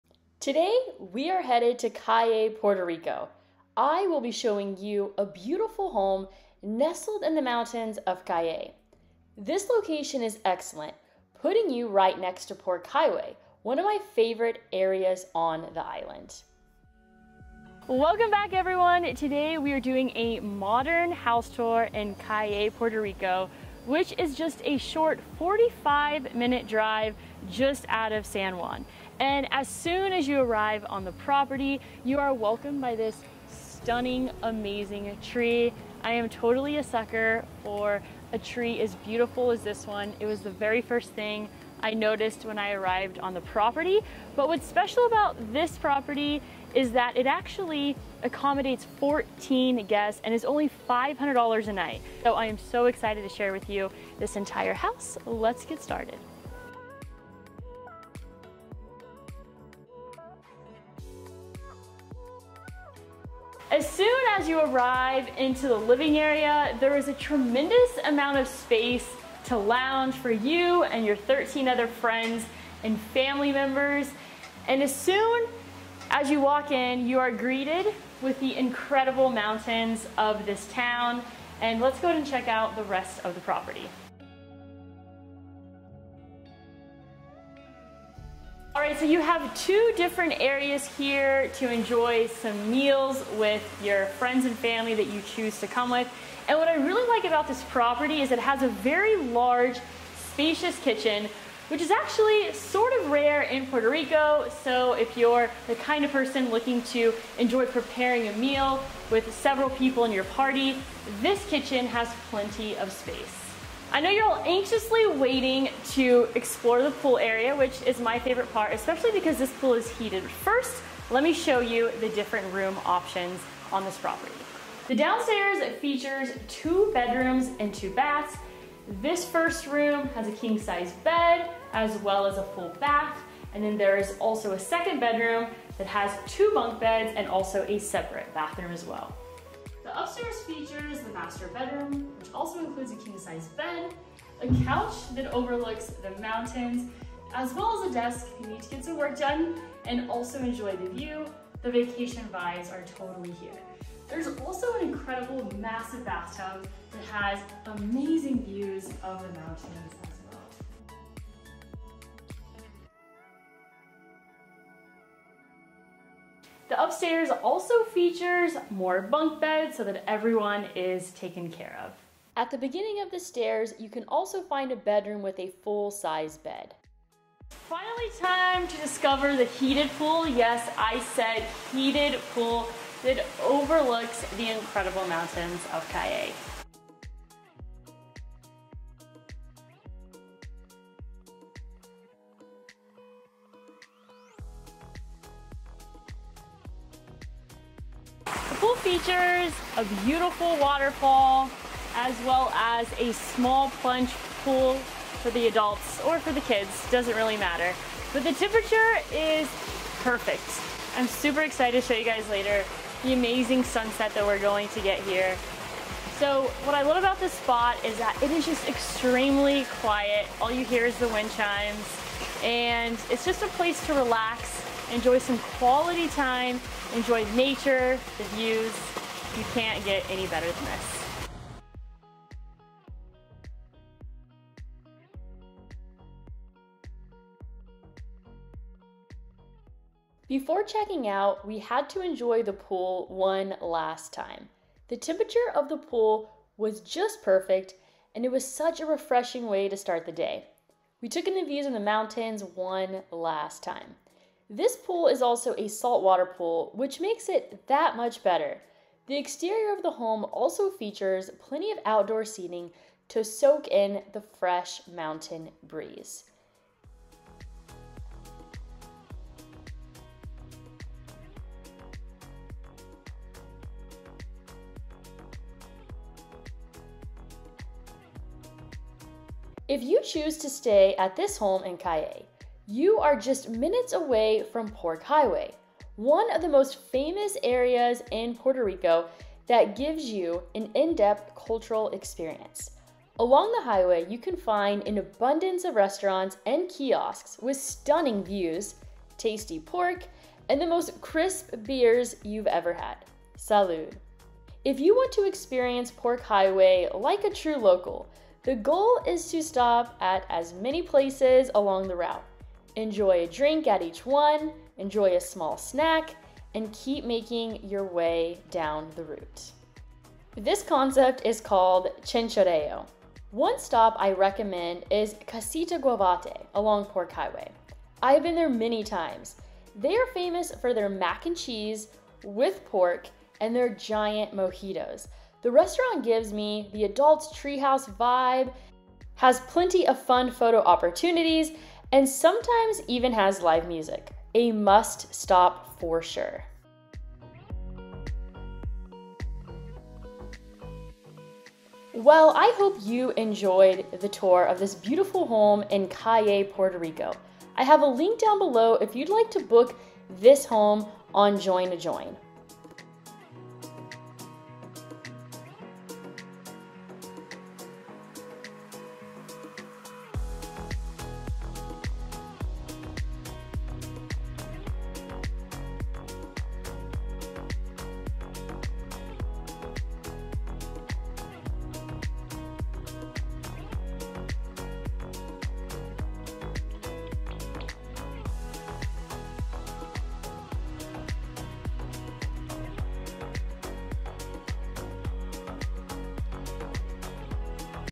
Check out this home in Cayey, Puerto Rico!
Beautiful and Modern House located amidst the mountains and surrounded by trees and vegetation that will allow you to enjoy nature in the Puerto Rico countryside.